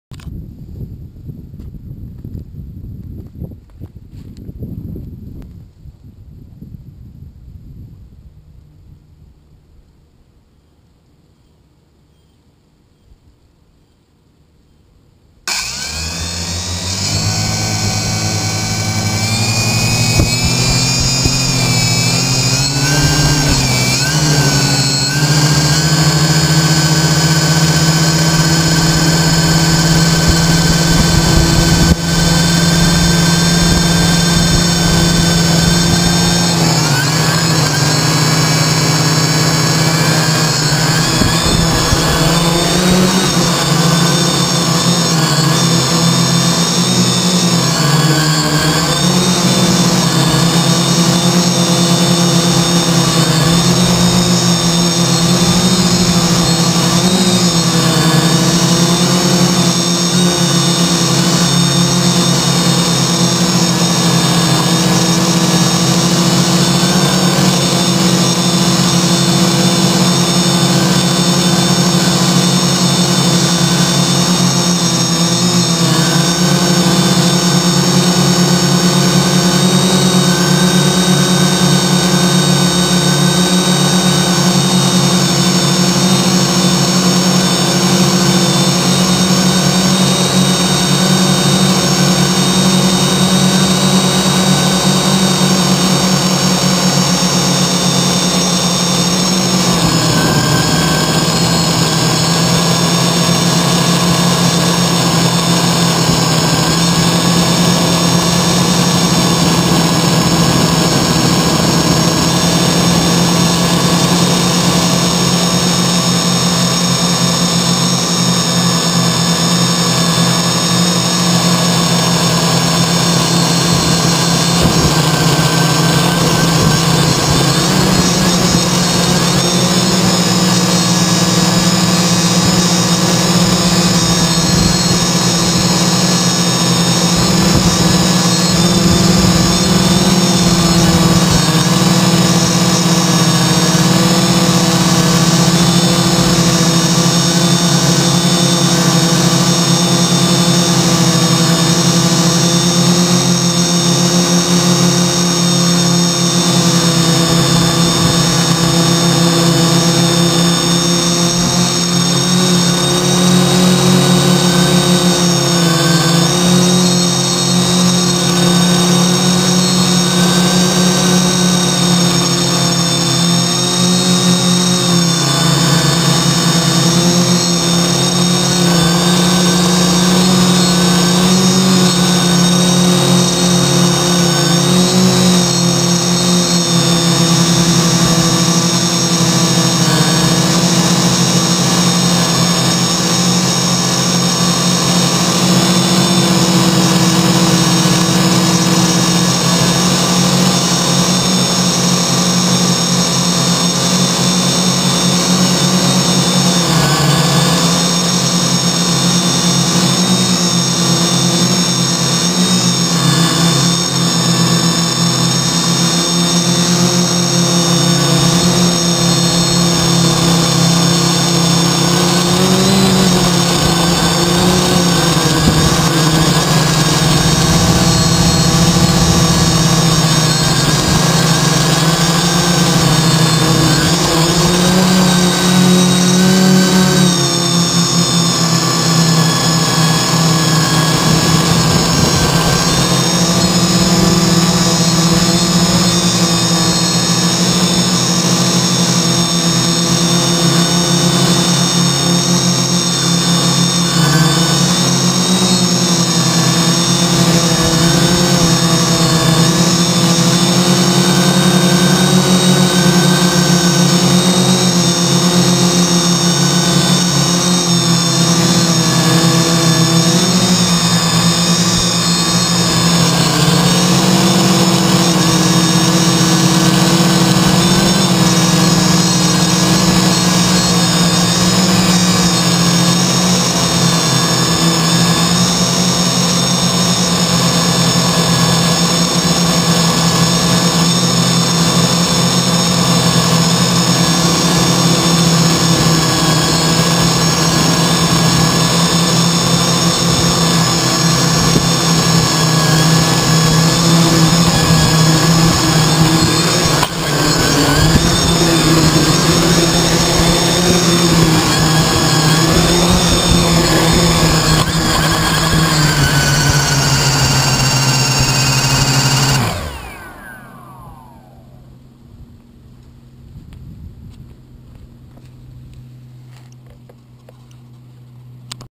CarbonCore Y6 first decent flight - GoPro 2 hard mount
My first decent test flight of the Y6 using a hard mounted GoPro 2.  Raw video.  720p 30fps.  It has some jello which is to be expected, but I just wanted to get some video.  I don't have the AG550 gimbal installed yet, until I get used to flying this copter.  I do not have the GPS installed either.  Still lots to learn.